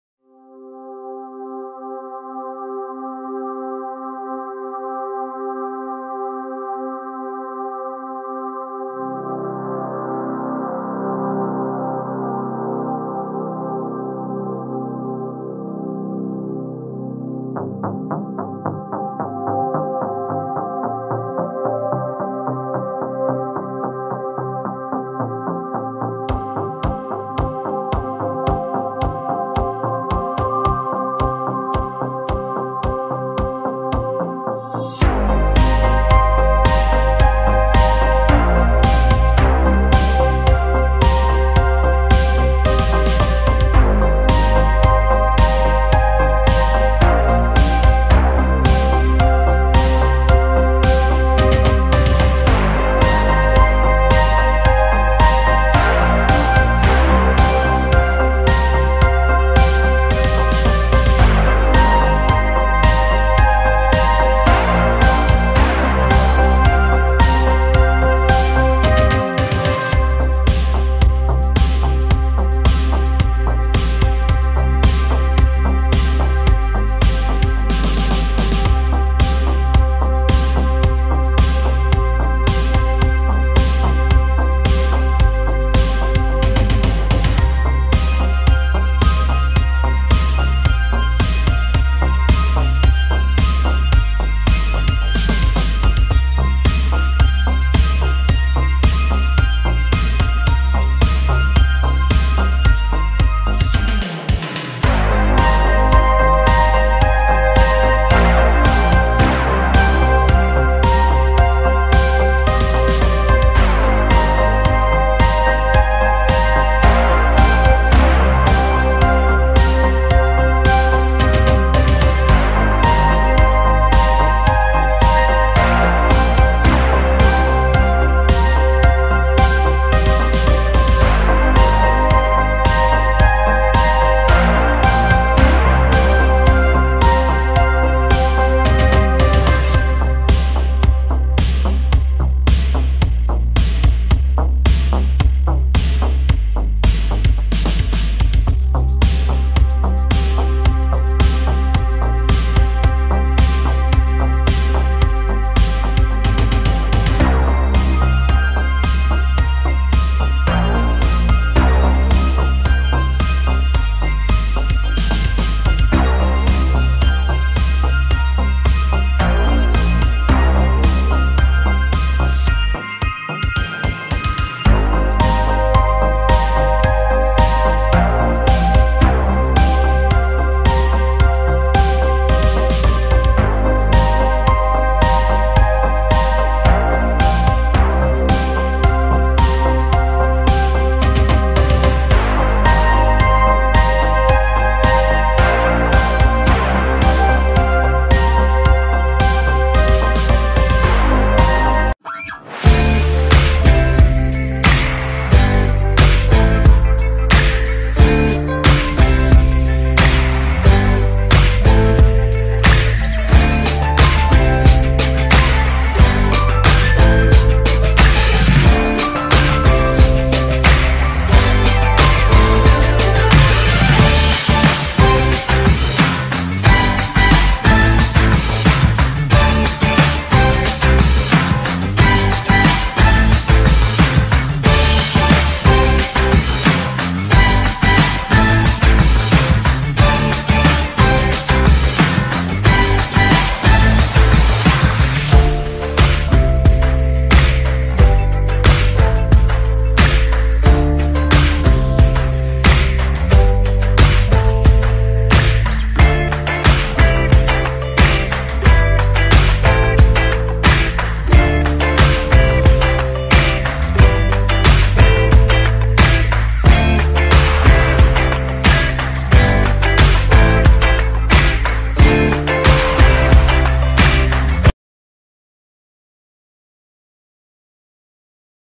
Super Mario Bros 3 drawing
An easy drawing of Mario from the classic video game Super Mario Bros 3, music by Karl Casey @WhiteBatAudio